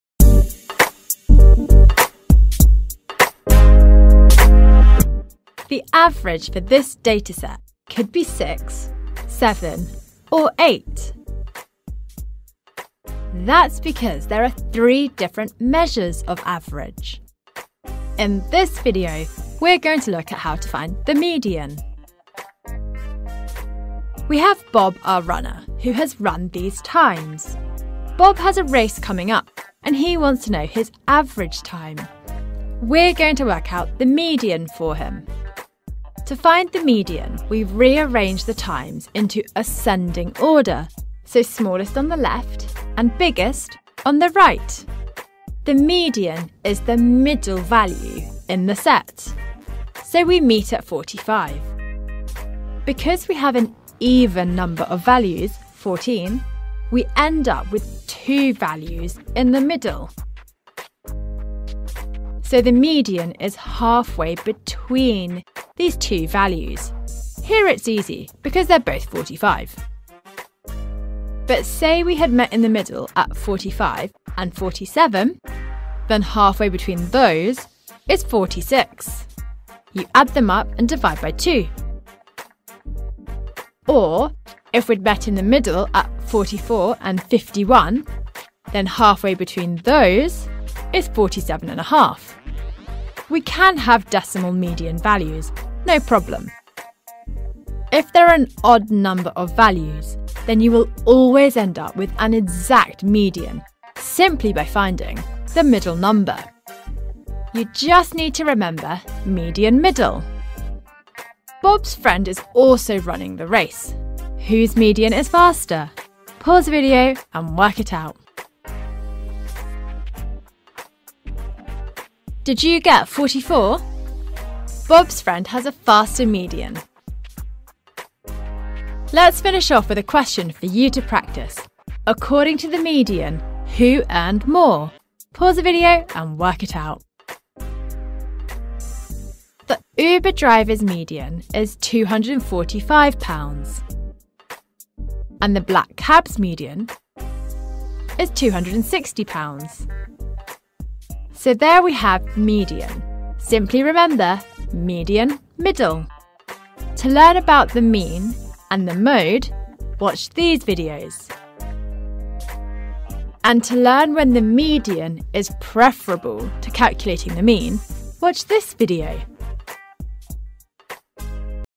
The Median | Probability | Maths | Zone 4
There are three different measures of average. In this video you are going to look at how to find the median.

To find the median, you re-arrange the given data into ascending order, so the smallest on the left - and the biggest on the right. The median is the middle value in a set.

Our teachers and animators come together to make fun & easy-to-understand videos in Chemistry, Biology, Physics, Maths & ICT.